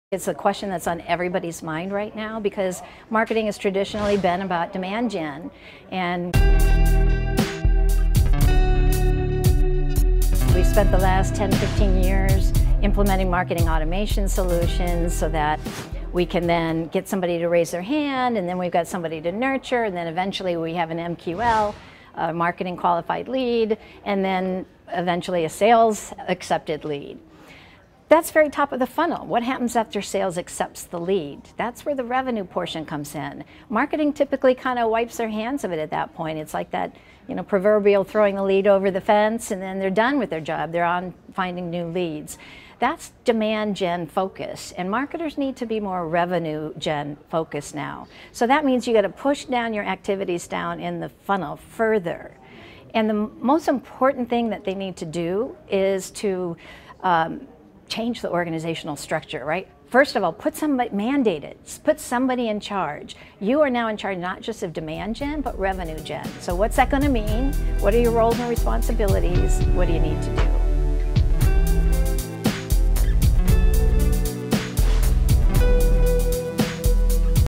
How can marketing before more revenue focused?
Nancy Nardin CEO at Smart Selling Tools discussed how can marketing before more revenue focused.